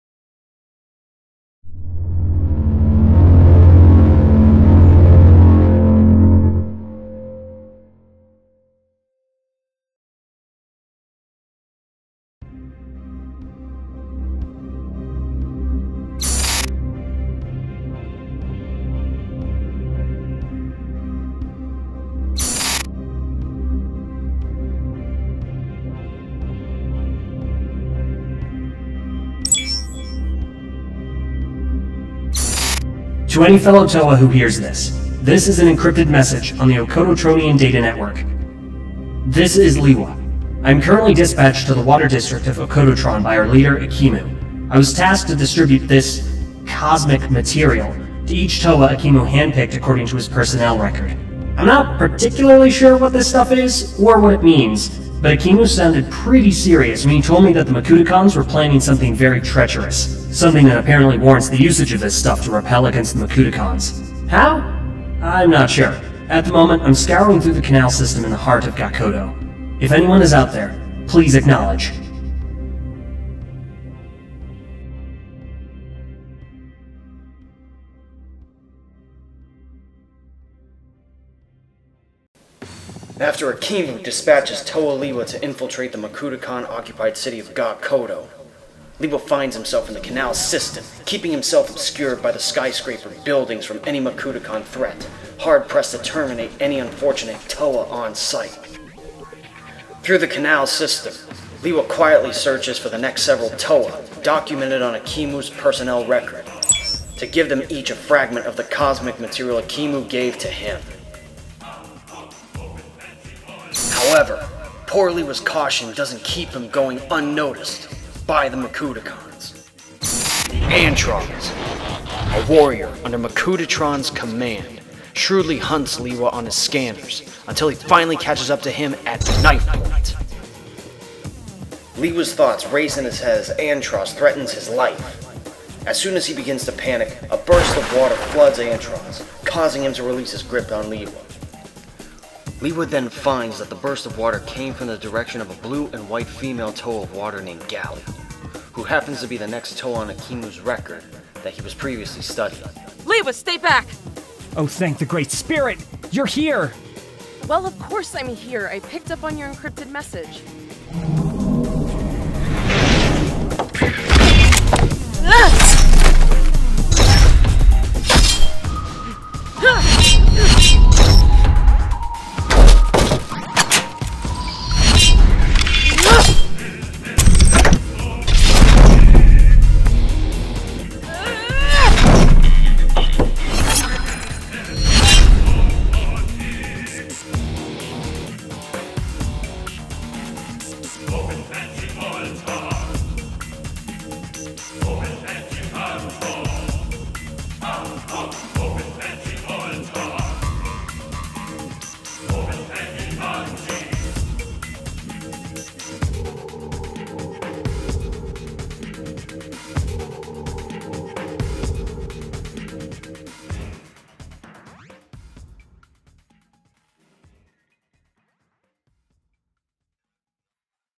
BIOFORMERS Ep 3 "Disembowel" Part 1 (BIONICLE Animated Series)
After being busy with online classes in the past as well as some other projects, I was able to finish this BIOFORMERS piece. This here is a new approach at expressing the story in the BIOFORMERS story. The 3d animating in maya on previous episodes takes a lot out of me so I'm looking to work on smaller animation projects. But at the same time I still have a story to tell. I think this animation comic hybrid might be a cool experiment to help with that.